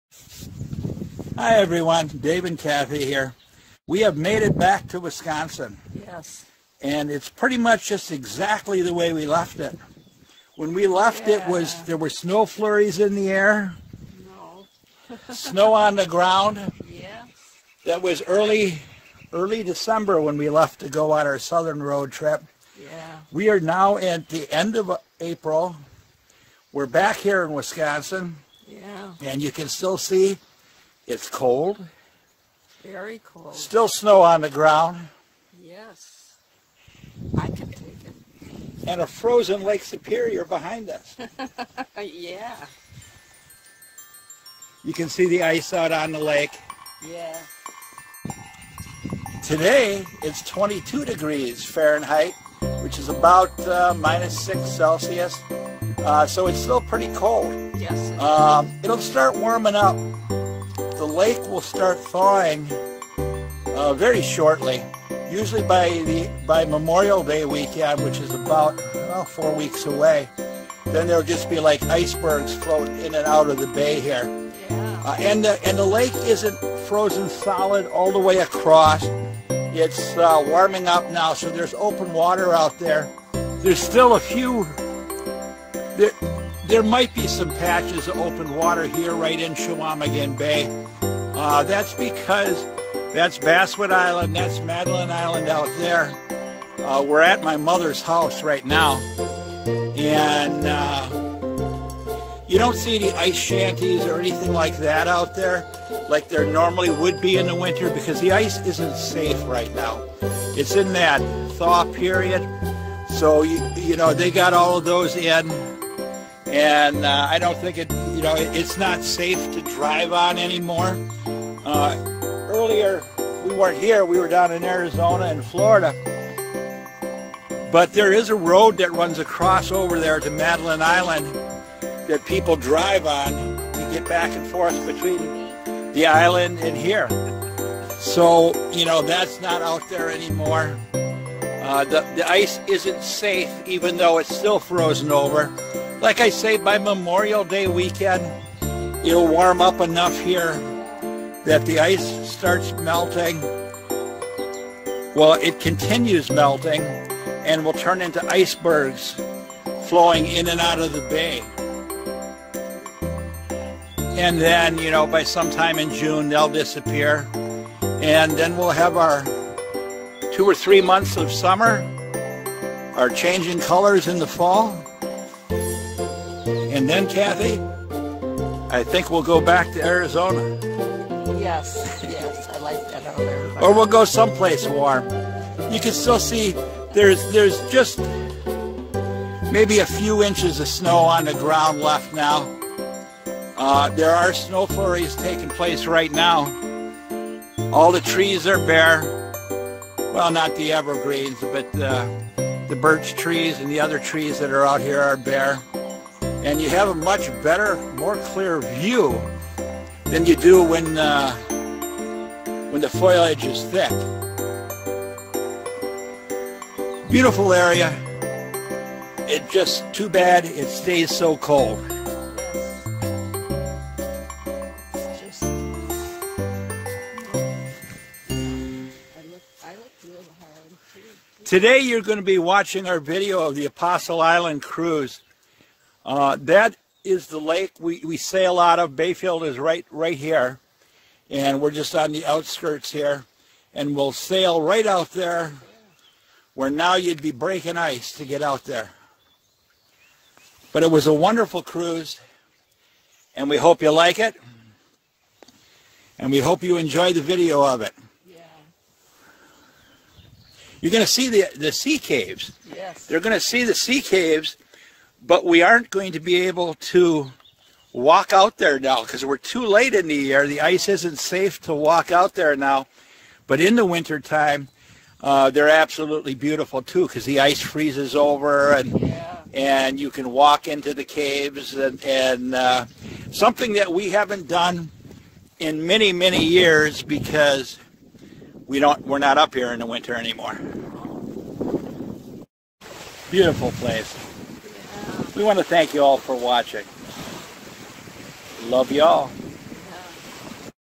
BACK IN WISCONSIN! IS THIS PARADISE?
Hi everyone. We have made it back to Wisconsin. Is this paradise? This video was recorded just this week. In our next video we're going to begin to finish our remaining videos from our 2021 Fall road trip. There's only a few left and then we'll begin our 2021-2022 winter road trip to Texas, New Mexico, and Arizona. Love you all!